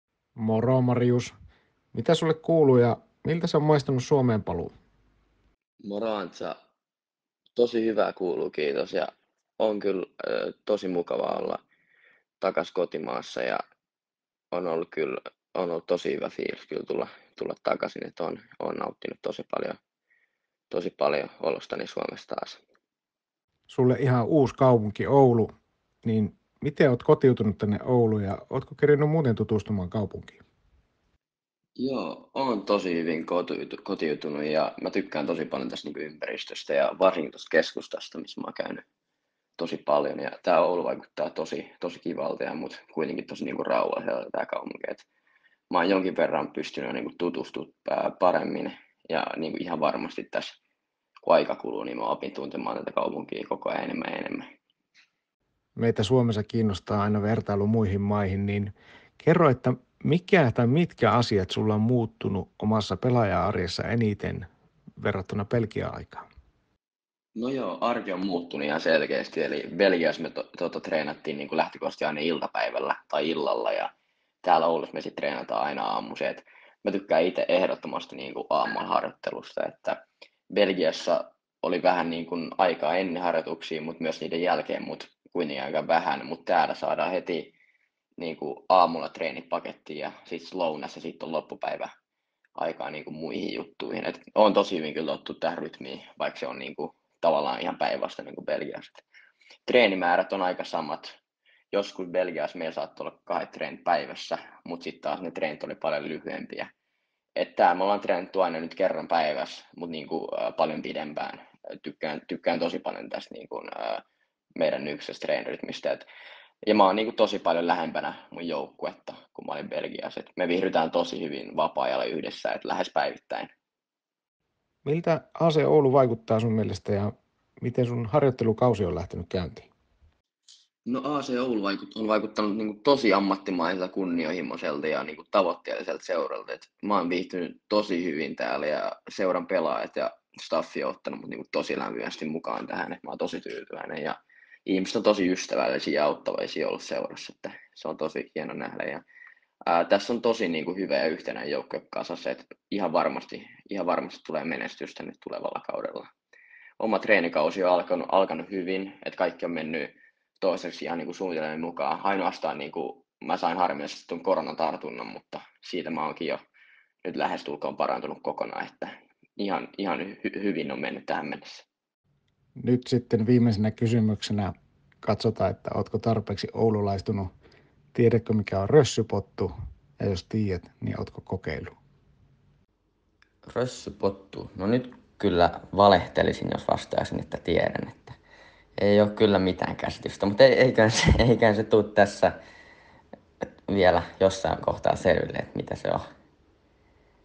ACOTV Extra: Pikahaastattelussa Marius Könkkölä
Tänään pikahaastattelussa kuullaan Marius Könkkölän kuulumisia ja millaista on ollut palata Suomeen.

Marius on Ekovirtauksen ja Laskukauppa.comin kummipelaaja.